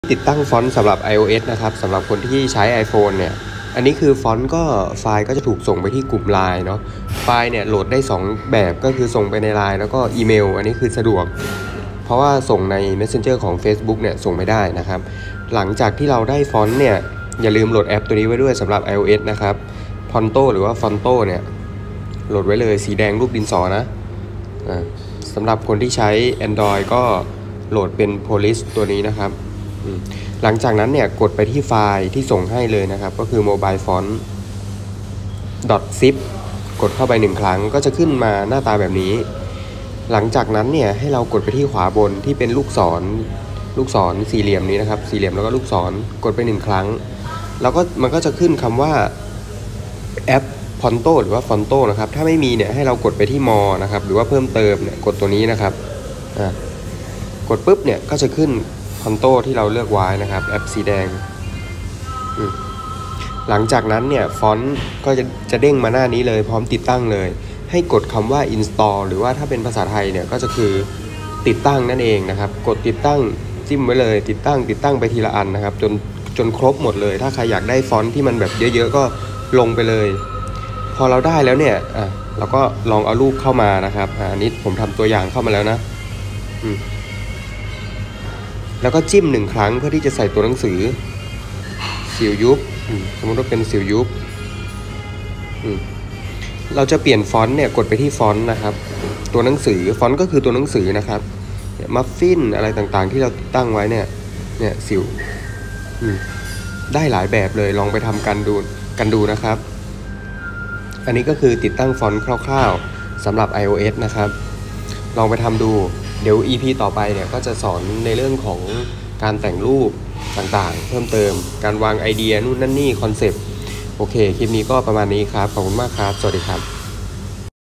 How to : ติดตั้ง font iOS iPhone iPad แอพ Phonto[สำหรับแม่ค้าออนไลน์ 2020]
แต่งรูปขายของบนมือถือ